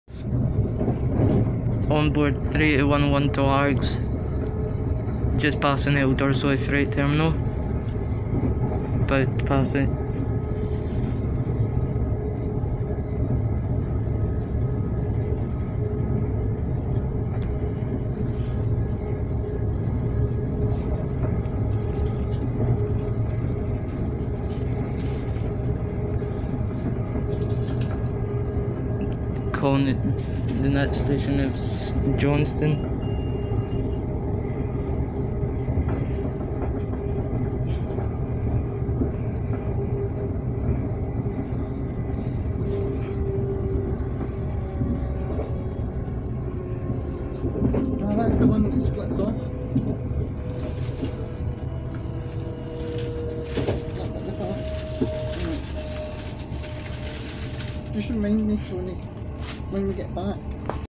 onboard a ScotRail trains class 380101 to Largs passing elderslie fright termal from the paisley
onboard the ScotRail trains class 380101 to Largs